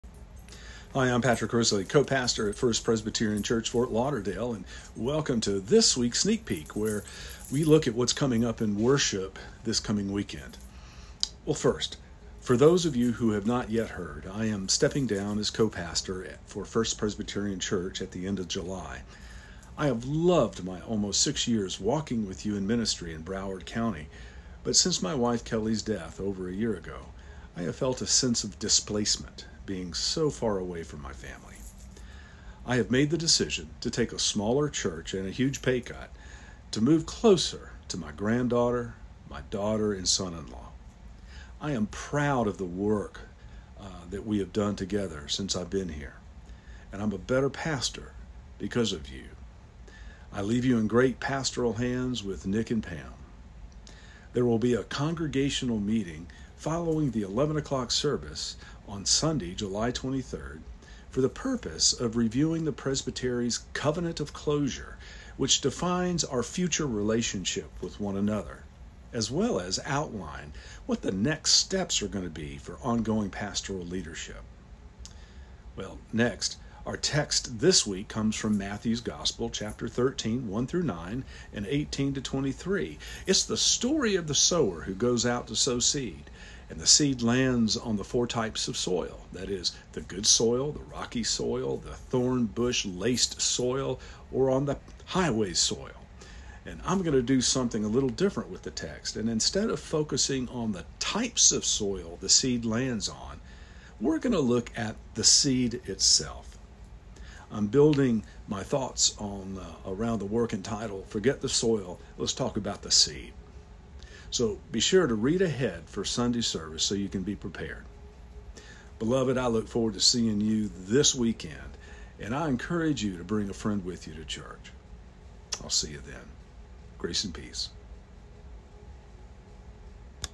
Pastor's Sneak Peek for Sunday, July 16, 2023.
A brief introduction into what is coming up for worship this weekend at First Presbyterian Church in Fort Lauderdale.